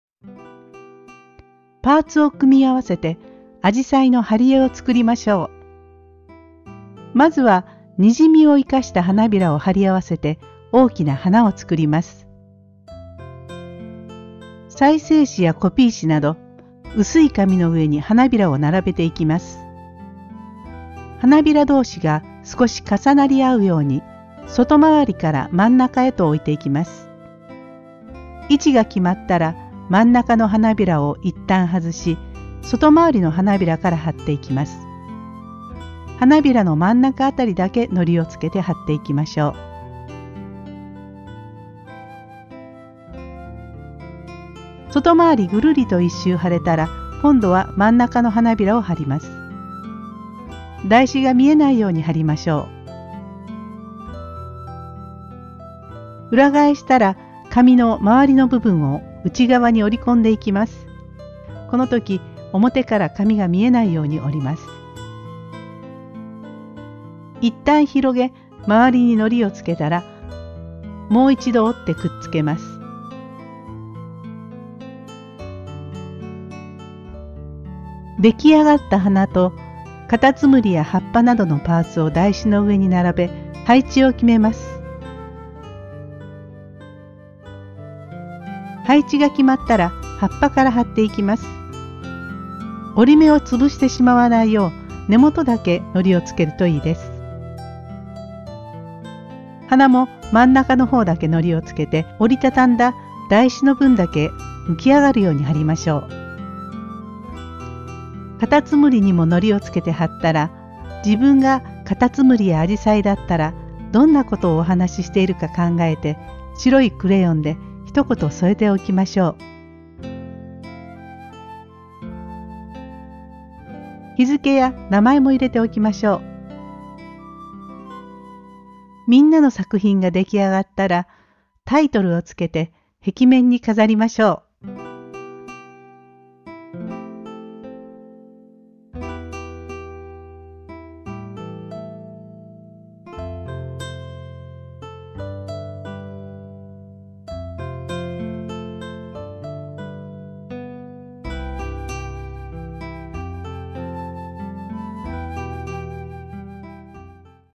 kimie gangi あじさいの花がさいたよ 貼り絵編
混色指導から、あじさいの花を作り、葉っぱやかたつむりと組み合わせて貼り絵にします。それぞれの動画はこちらをご覧ください。